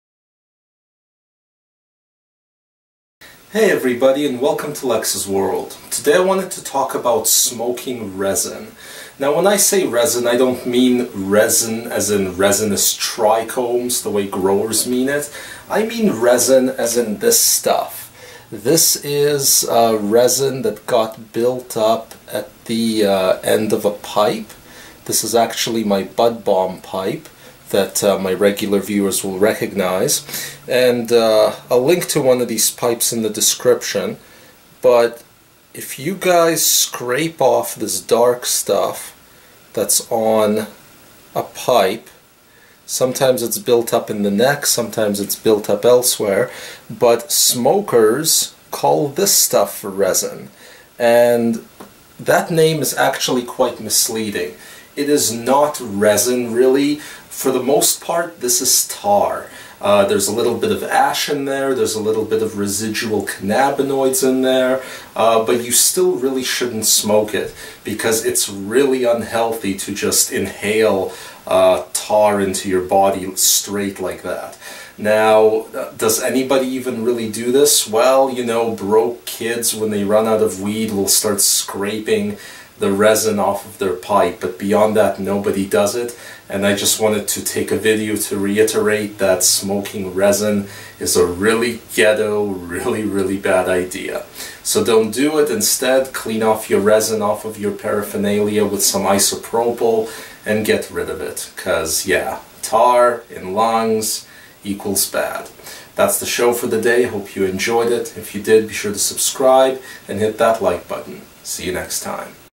SAFETY TALK - Smoking Pipe Resin – Black Cannabis Tar
Today on Lex’s world let’s discuss Marijuana Pipe Resin; and why you should NOT smoke it. In brief; it’s not ‘resin’, really...that’s just a term that stuck. Largely, it is a mix of tar, ash and residual cannabinoids...often enough to get a bit high, but it’s not worth inhaling this! Smoking tar is a desperate act usually done by people who are super dry or teens...plus there’s horror stories of those who vomited HARD after smoking pipe resin or experienced pain in their lungs for hours.

There used to be a direct link to a pipe here, but YT doesn't allow that anymore. BUT I do have a grow gear and head shop on amazon